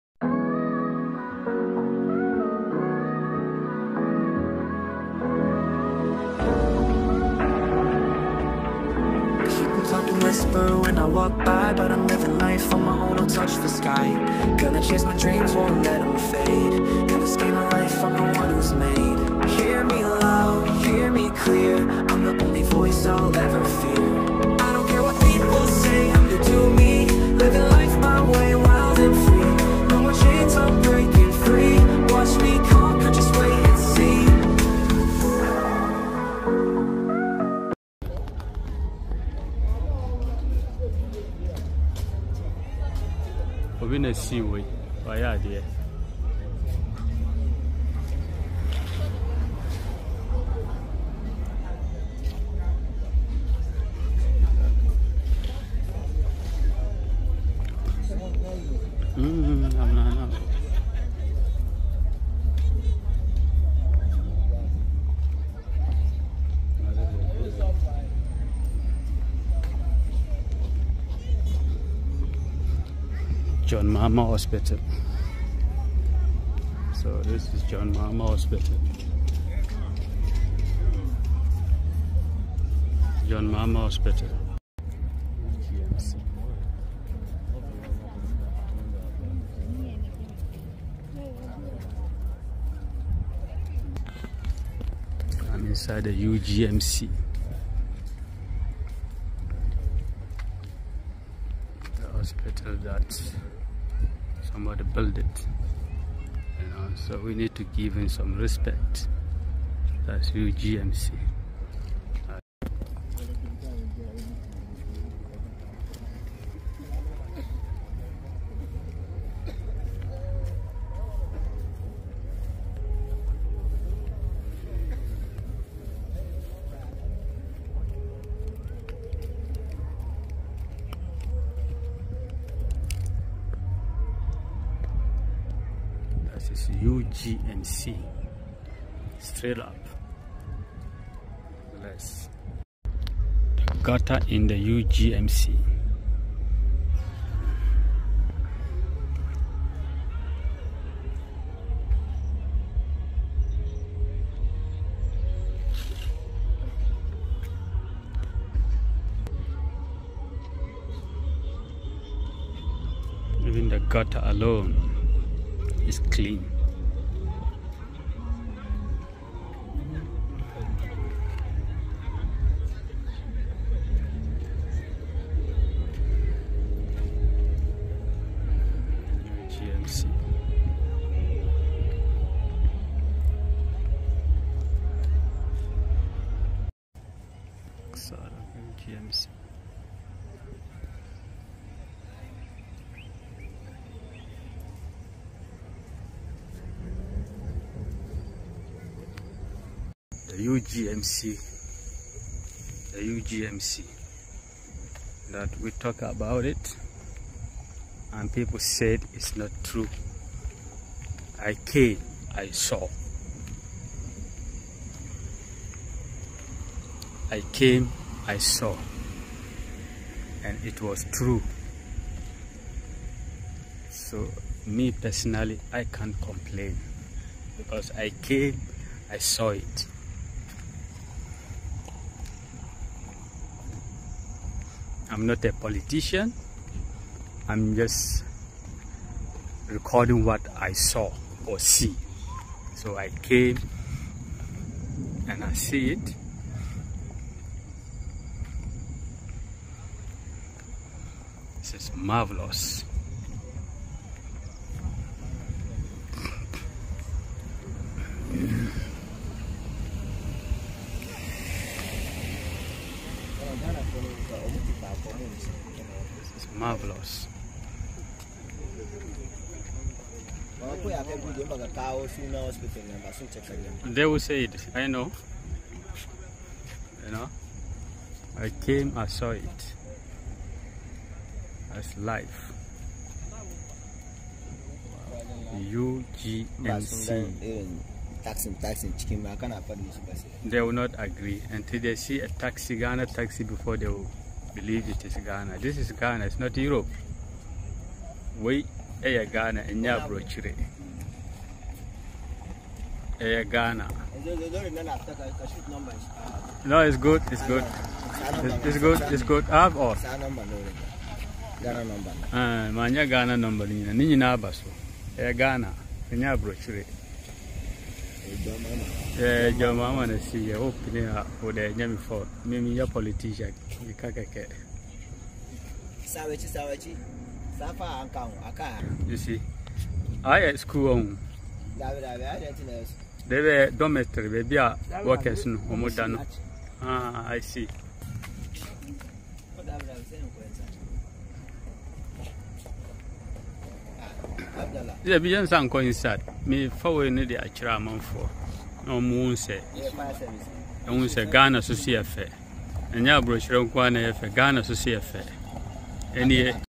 The UGMC Hospital in Accra.
This hospital is one of the top hospitals in Ghana.